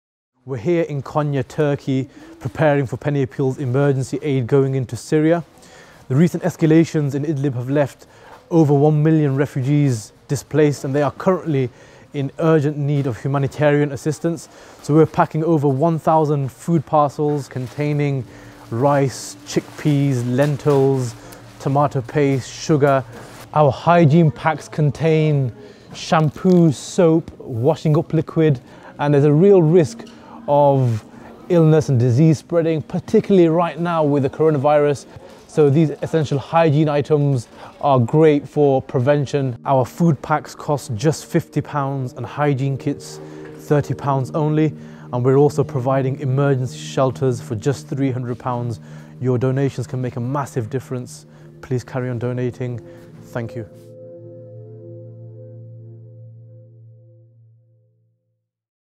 Syria Emergency
Alhamdulillah, just landed back in the UK!! Today marks 9 years of conflict in Syria. We are now facing an entire generation of children who have known nothing but war. Every child - no matter where they're from - deserves the right to a childhood. Being a father myself it's been overwhelmingly heartbreaking visiting refugee families on the Syria border and seeing how much they're still suffering. I just couldn't imagine my own daughter going through that. What father could!? 😥

Watch the video to see how we've been packing essential aid for the Syrian refugees. I've pledged to raise £25,000 for Penny Appeal's Emergency Response programmes. I'll be running 3 World Major Marathons this year, insha Allah as part of my next BIG challenge -

The threat of the coronavirus is leaving hundreds of thousands of Syrians extremely vulnerable to this virus - hygiene kits are needed now more than ever.